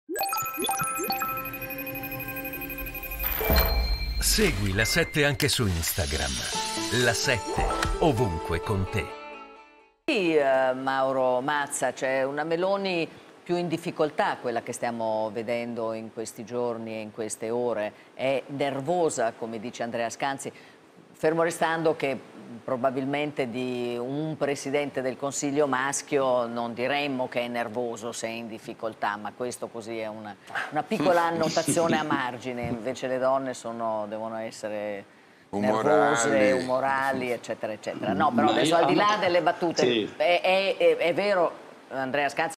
Meloni, Lilli Gruber: "Di un premier maschio non diremmo che è nervoso se è in difficoltà..."
Giorgia Meloni, Lilli Gruber: "Di un Presidente del Consiglio maschio non diremmo che è nervoso, se è in difficoltà. Invece le donne devono essere umorali..."